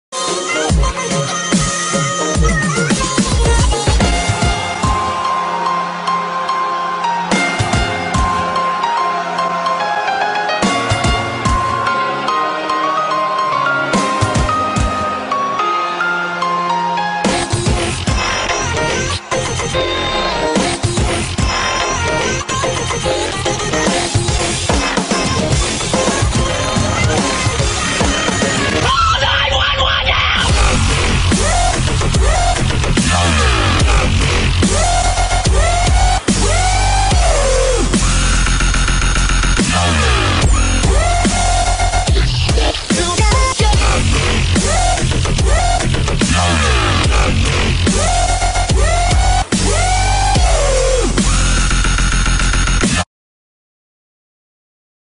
Flip Practicing with DUBstep.
A couple friends practicing flips at a gym.